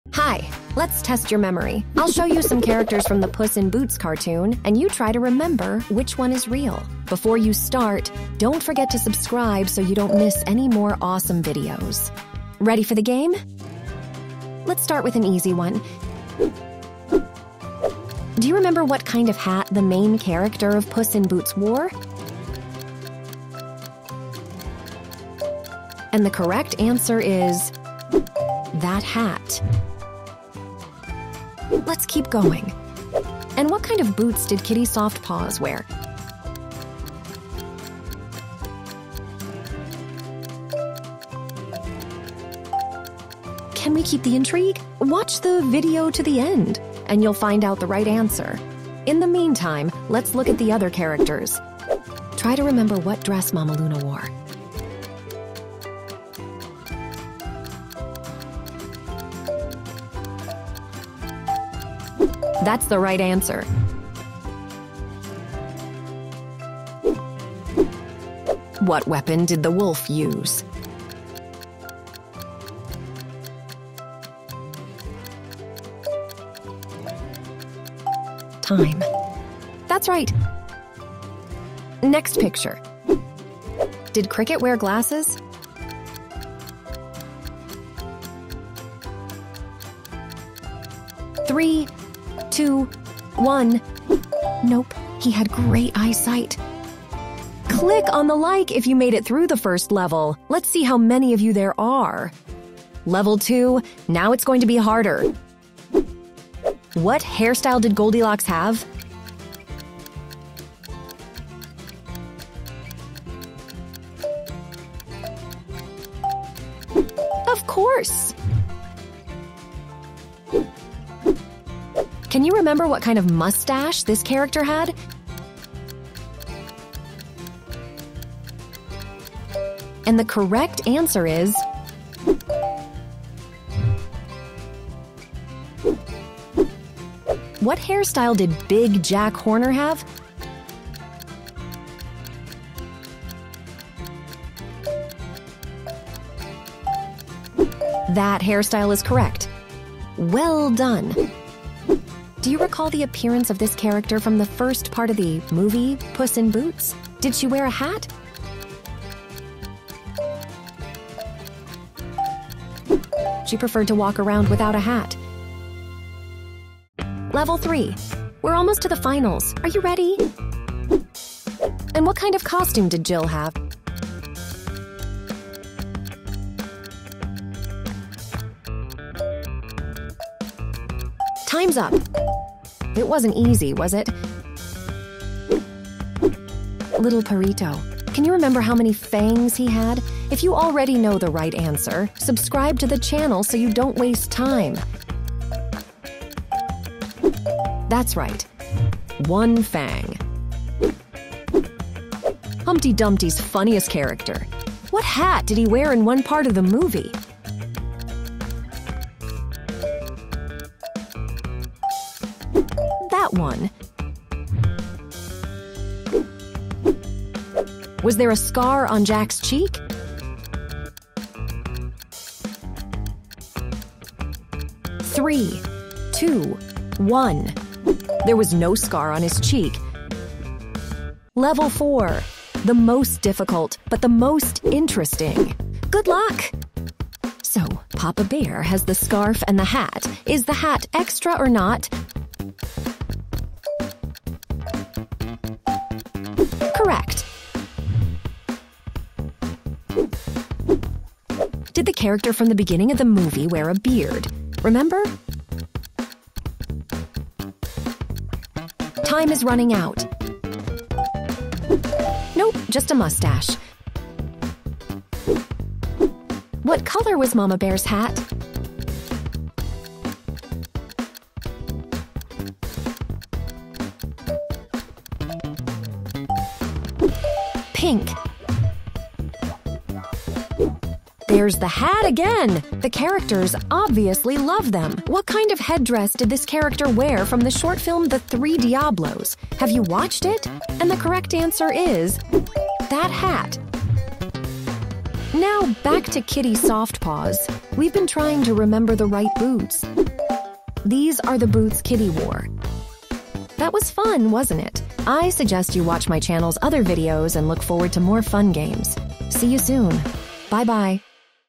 Not everyone can do it! Guess the Real PUSS IN BOOTS Character
In this quiz, try to guess the real character from the DreamWorks cartoon PUSS IN BOOTS the last wish! Not everyone can do it, try checking your memory!
I'll show you two pictures, and you pick the right answer. For each correct answer, you get one point!
After watching the video, be sure to tell me your results in the comments, it will be very interesting to read, as well as write which character was the hardest to guess!

If this is your first time on my channel, I invite you to play other fun games.